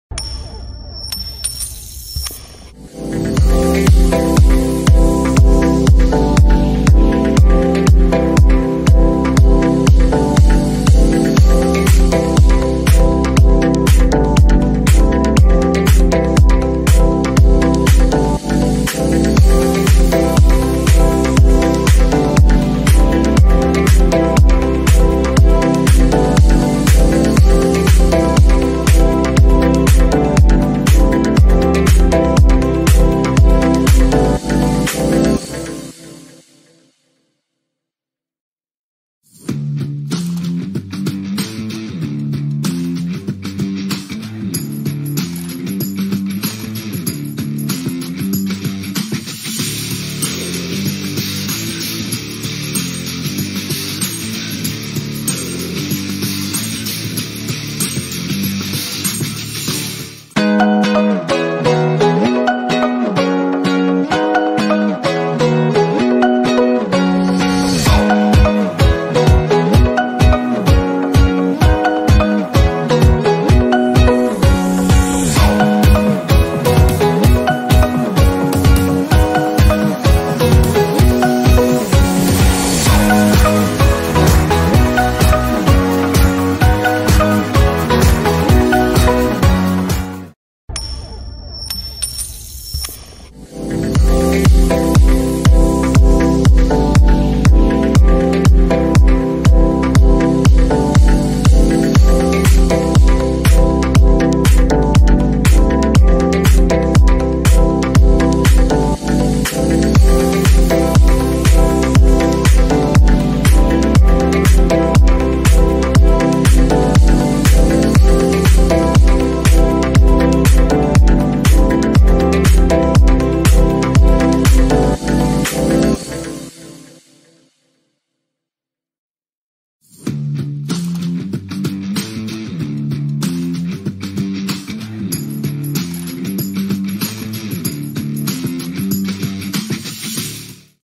Electric scooter problem and solution ¦¦ Electric converted scooter Ignition not working
Ignition of electric converted activa scooter was not working. Problem was detected and solved it temporarily to get scooter working. But need to change ignition for permanent solution.

This is just a temporary solution to get scooter running..